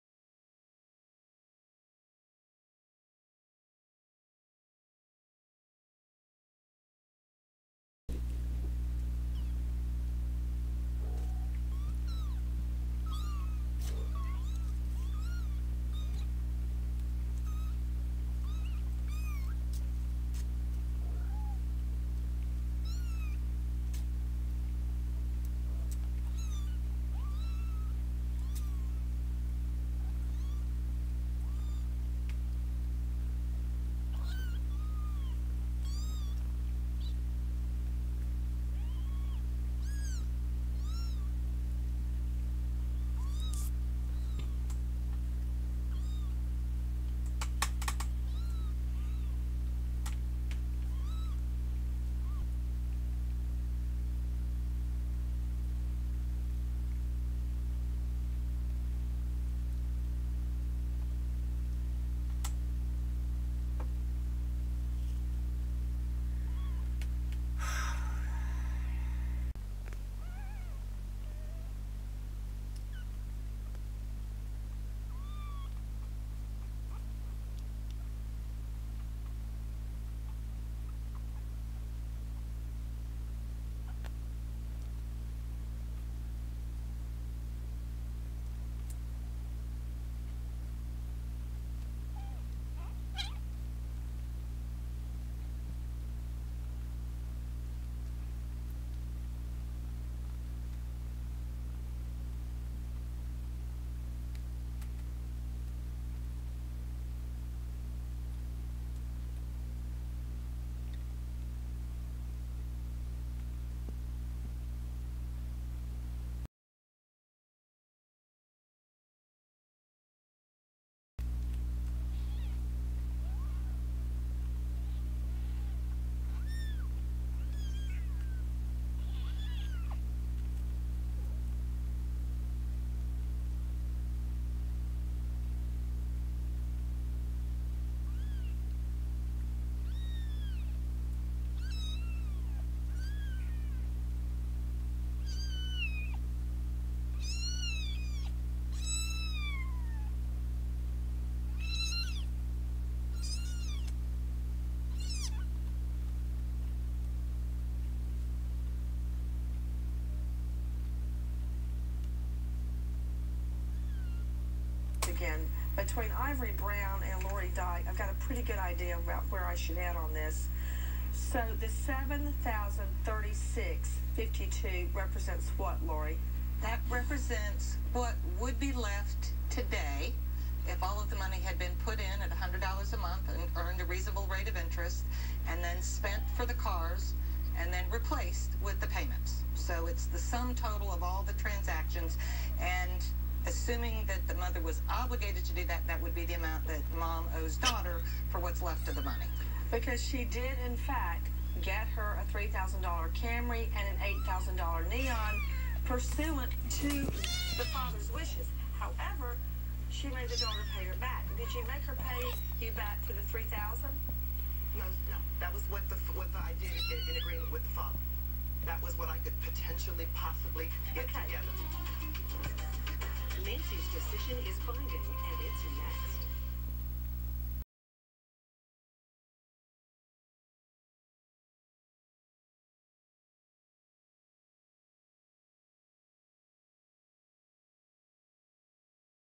Kitten fight
Two kittens have been battling since day 2. To add to the noise, there's a big sister who keeps sticking her nose in. Sometimes to play mommy and sometimes to play baby. I think she's psychotic.